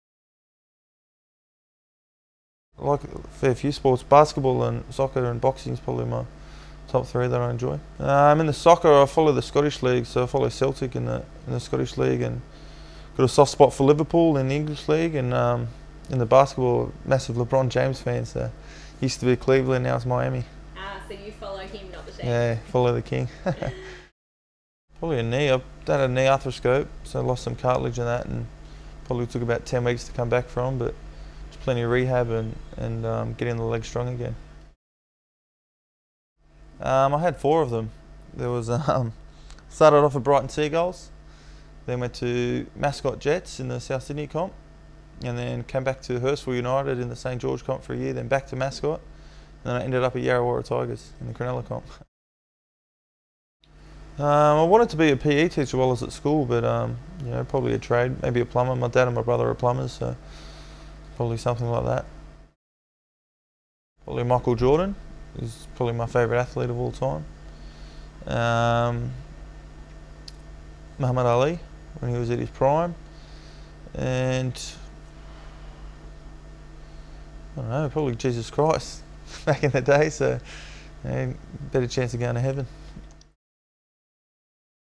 Keith on the Couch
Keith Galloway chats with Footy Social about soccer, injuries and the fast track to heaven!